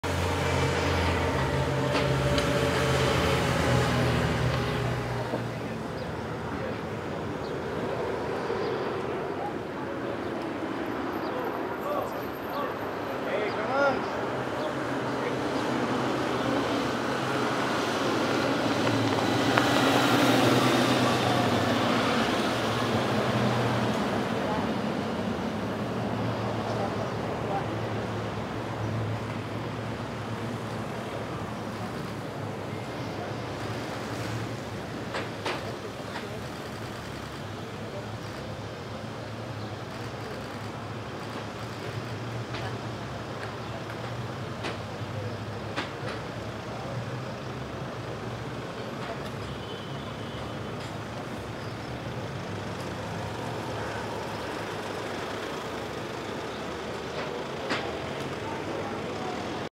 L'arrivée de Joe Biden au parc La Grange, à Genève
Sommet Biden-Poutine, le convoi du dirigeant américain arrive au parc La Grange où va se dérouler sa rencontre avec Vladimir Poutine.